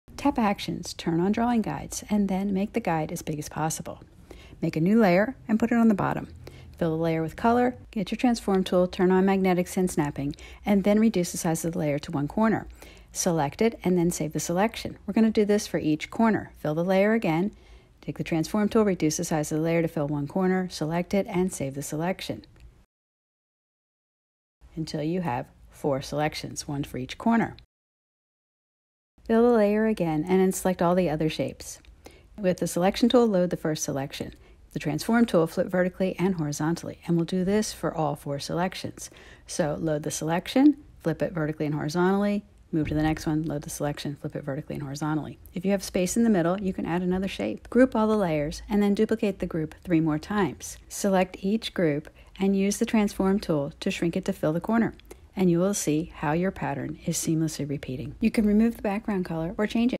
Procreate tutorial - seamless pattern (YouTube )
A quick easy tutorial showing how to make a seamless pattern in Procreate. Seamless patterns are great for fabric design and surface design projects. They are also useful for backgrounds and filling objects in your Procreate drawings.